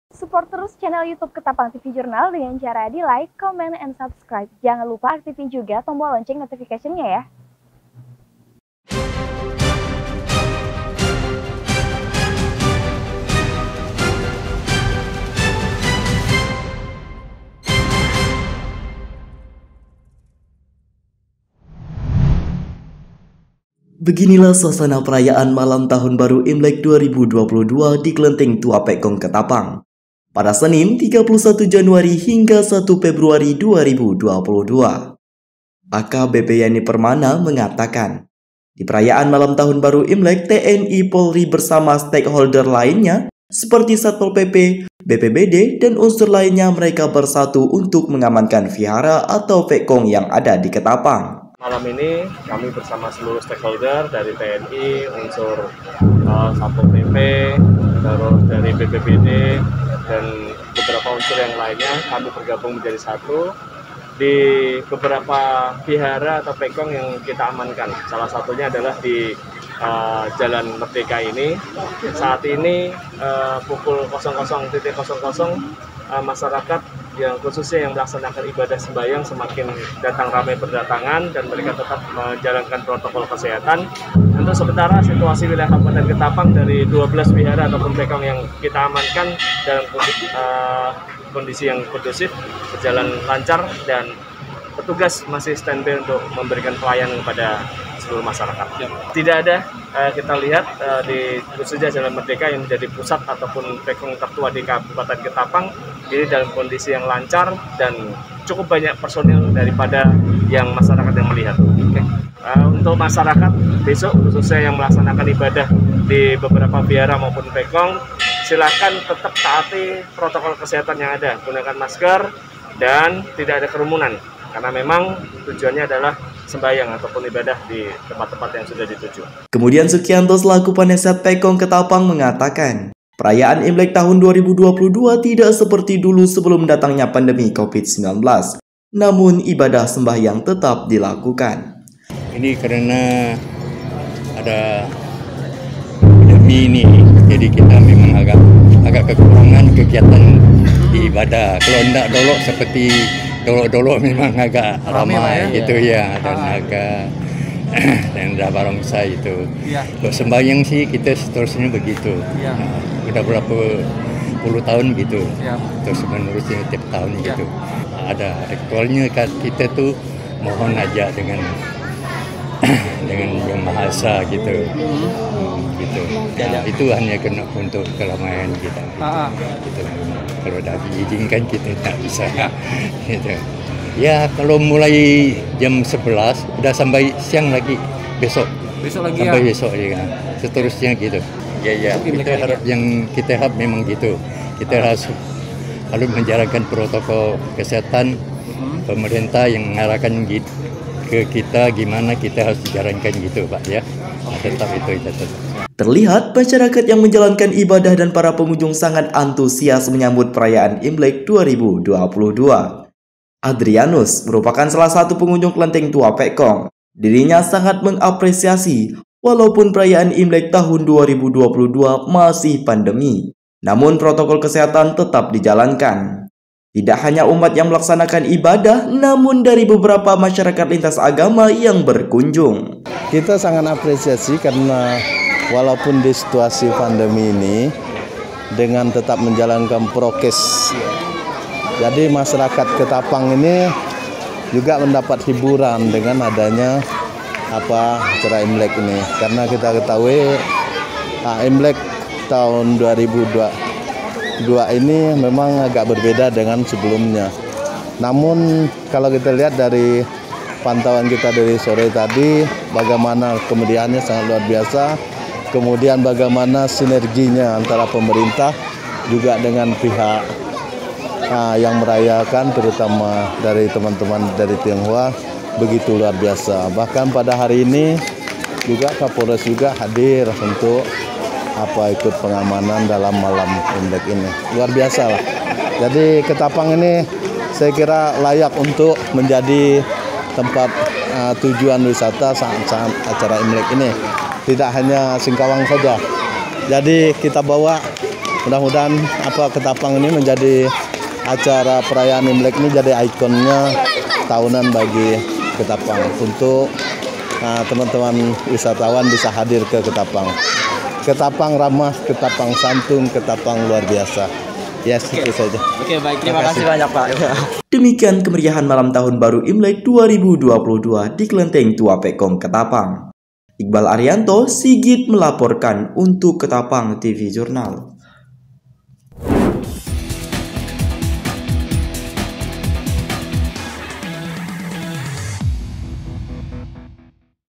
SUASANA MALAM PERAYAAN IMLEK 2022 DI KELENTENG TUA PEK KONG KETAPANG
BEGINILAH SUASANA PERAYAAN MALAM TAHUN BARU IMLEK 2022 DI KELENTENG TUA PEK KONG KETAPANG PADA SENIN 31 JANUARI HINGGA 1 FEBRUARI 2022.
AKBP YANI PERMANA MENGATAKAN, DIPERAYAAN MALAM TAHUN BARU IMLEK TNI, POLRI BERSAMA STEAK HOLDER LAINNYA SEPERTI SAT POL PP, BPBD DAN UNSUR LAINNYA MEREKA BERSATU UNTUK MENGAMANKAN VIHARA ATAU PEK KONG YANG ADA DIKETAPANG.